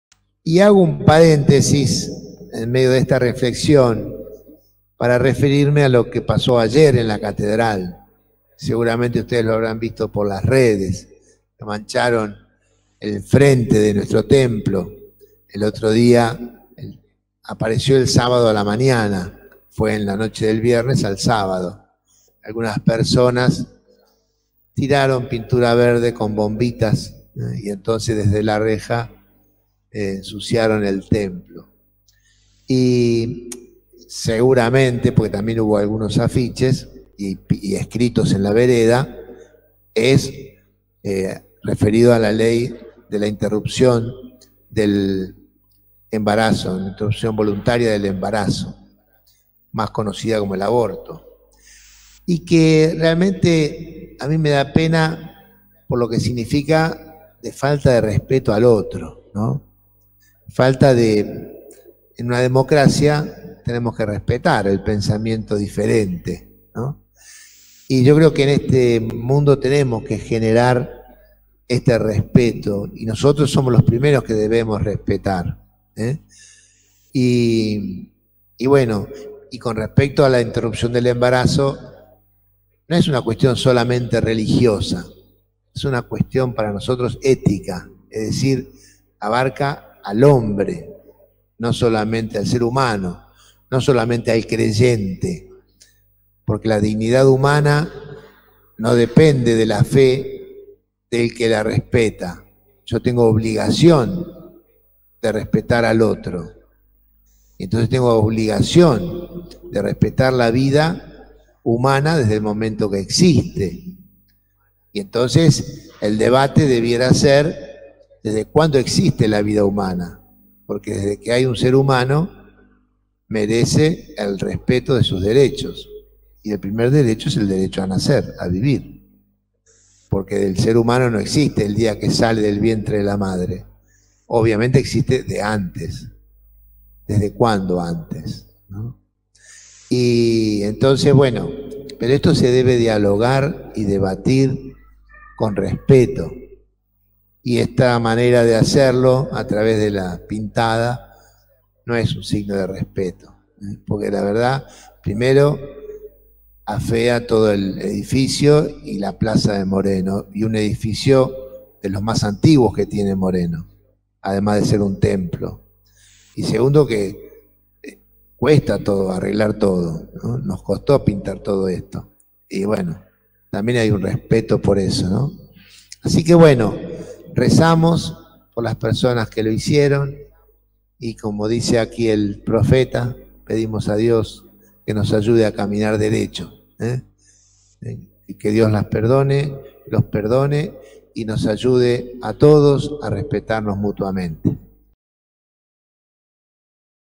En la misa de este domingo, el padre Fabián Sáenz se refirió a las pintadas verdes en la catedral.
Pintadas verdes en la catedral de Moreno: En la misa del Primer Domingo de Adviento, el padre Fabián Sáenz se refirió al repudiable hecho.